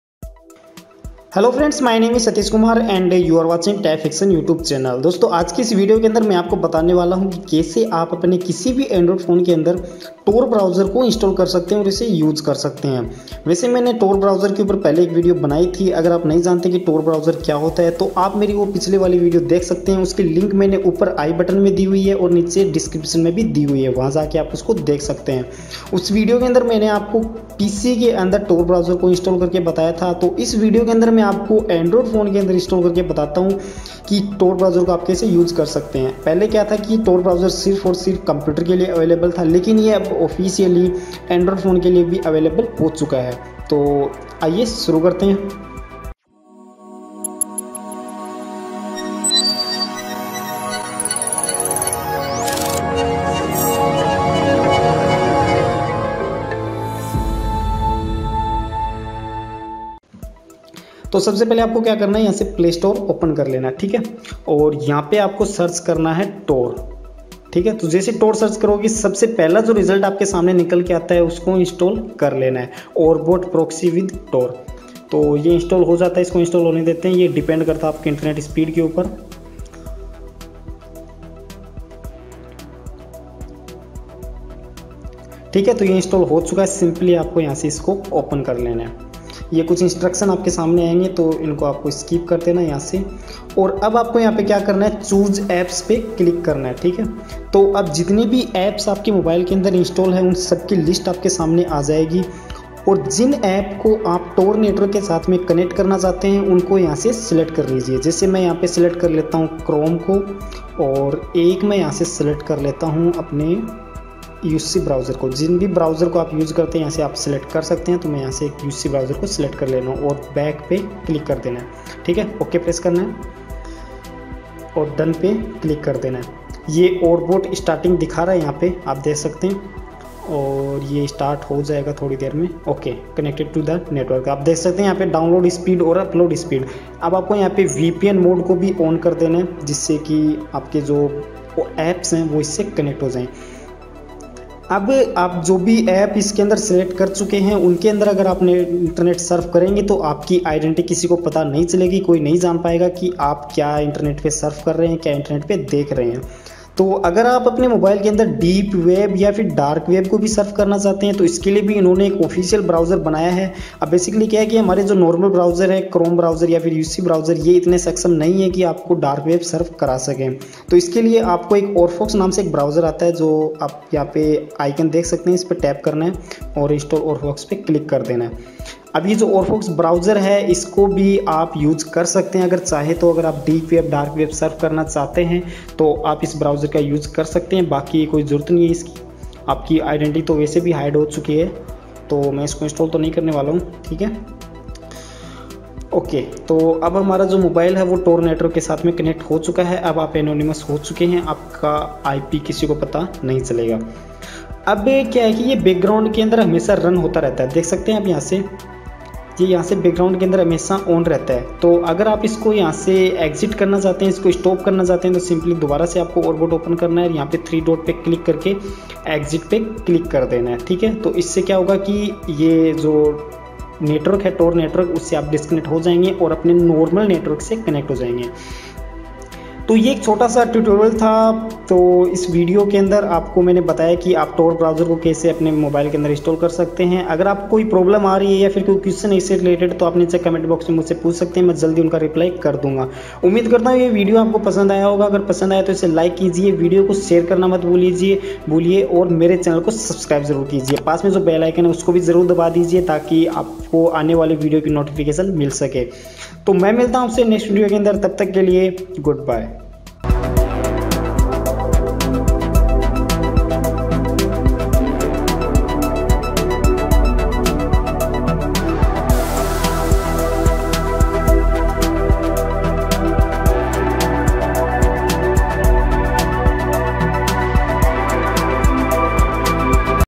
How To install Tor Browser on any android phone.? Now Surf deep or dark web on android
Hello Everyone!
My Name is Satish kumar and welcome to the tech fiction channel

Is video me main aapko bataunga ki kaise aap apne android smartphome me tor browser use kar sakte hai aur apni ip hide kar sakte hai. aasha karta hu apko ye video pasand ayegi. is video ko like kare aur apne dosto ke sath share kare.

Thanks.

What is Tor Browser.? How To Download and install on PC in Hindi